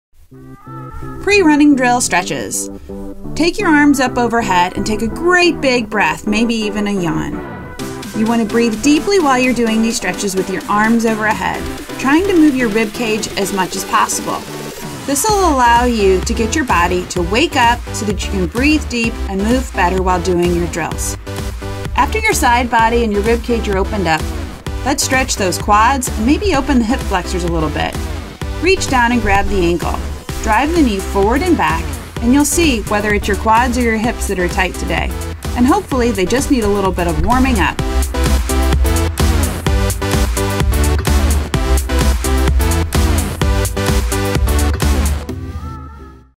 Essential Running Drills - Pre Drill Stretches
Whether you’re training for a marathon or your first 5K, you can benefit from running some drills. I’ll show you my favorite drills in the playlist linked below.